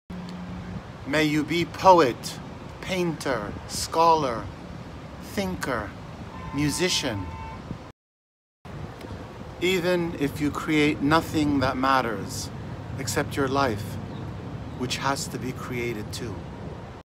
The Art of Living 🙏🏼✨By Nissim Ezekiel
Blessings

 MMay you be
Poet, painter, scholar,
Thinker, musician,
Even if you create nothing that matters
Except your own life, which too
Has to be created

May you read
Wisdom books
In the spirit of comics
And the comics
In the spirit of wisdom books

— Nissim Ezekiel

Nissim Ezekiel, a renowned Indian poet, was born in 1924 to an Indian Jewish family and passed away in 2004.

His work often delves into themes of religion, philosophy, and human experiences.

Ezekiel's poetry, including his "Latter-Day Psalms," showcases his unique blend of humor, tolerance, and broad-mindedness.

He infuses his work with influences from Hindu traditions and individual talent, creating a body of work that reflects religious strains in an unconventional manner

Despite his Jewish background, Ezekiel's poetry demonstrates a deep connection to India and spiritual themes, highlighting his versatility as a poet and his exploration of philosophical and existential aspects of life

Moreover, Ezekiel's impact extended beyond poetry; he held various roles such as an Advertising Manager, Vice-Principal of a college, teacher, editor, playwright, and columnist.

His contributions earned him accolades like the Sahitya Akademi Award and the Padma Shri Award. Notably, Ezekiel's work resonated with influential figures like Shimon Peres, who admired his poetry for its profound messages of peace and introspection

Nissim Ezekiel, an Indian-born poet of Jewish descent, is often referred to as the "father of post-independence Indian verse in English." His poetry reflects a unique blend of themes, styles, and influences that have left a lasting impact on Indian literature.

Ezekiel's work is characterized by its simplicity, sharpness, and austere style, often devoid of mythological elements or elaborate poetic devices. He is known for his sharp observations of human relationships, everyday life, and societal issues, presented with wit, sarcasm, and irony

Ezekiel's poetry delves into a wide range of subjects and themes, focusing on the human predicament, the quest for identity, and the essence of humanity.

His poems often explore the complexities of human emotions, ethical dilemmas, and the search for meaning in a rapidly changing world.

Through his verses, Ezekiel conveys a sense of humanism that emphasizes dignity, moderation, and the rejection of dogma and superstition

Moreover, Ezekiel's poetic career evolved from his early dreamy style to a more truth-seeking and socially conscious approach.

He addressed issues of corruption, disenchantment, and societal challenges in India while advocating for positive change through his poetry. Despite facing criticism for his writing style and themes that were perceived as too closely aligned with colonial influences, Ezekiel remained dedicated to expressing the truth and highlighting the ordinary aspects of human life with vibrancy and depth

In summary, Nissim Ezekiel's poetry is a reflection of his deep love for India, his exploration of spiritual themes, and his commitment to portraying the warmth of human relationships through vibrant language. His works continue to be studied and appreciated for their insightful commentary on society, human nature, and the pursuit of humanity through ordinary means.